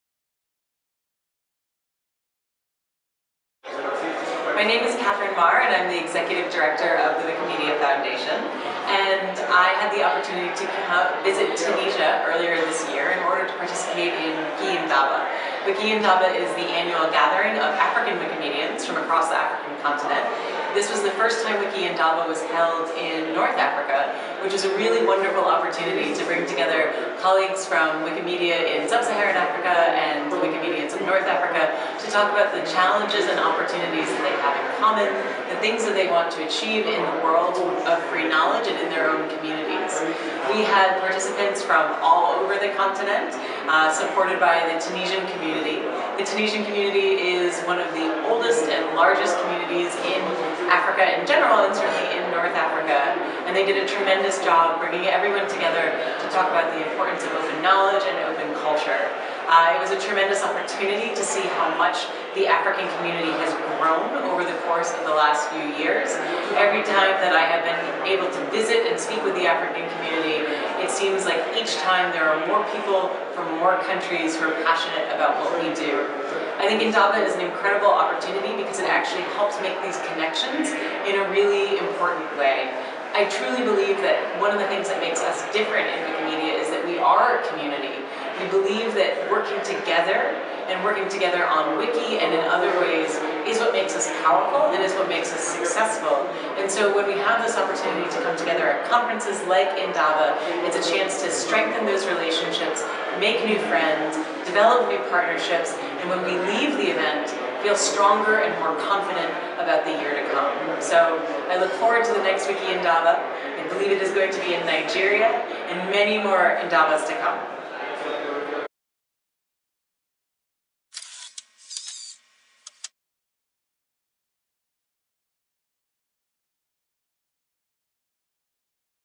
Interview Katherine Maher WikiIndaba Tunis
Interview with Katherine Maher, Executive director of the Wikimedia foundation about the WikiIndaba conference held in Tunis in March 2018.

Video filmed in Berlin 21 april 2018 and edited by Habib Mhenni